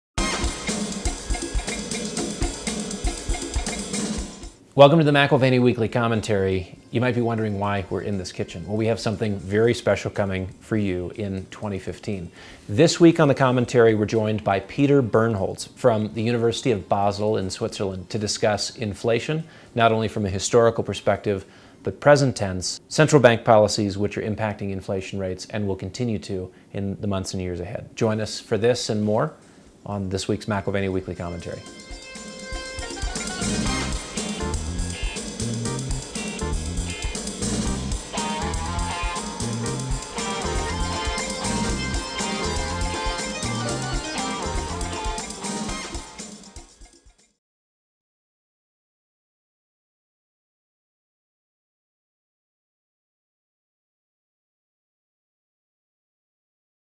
Peter Bernholz Hyperinflation | Politicians have an Inflationary bias
Pre 1914, Gold Standard restricted Inflation
Almost all hyperinflations occurred after 1914
Politicians have an Inflationary bias

► Getting Away with Spending 4 times what You Make!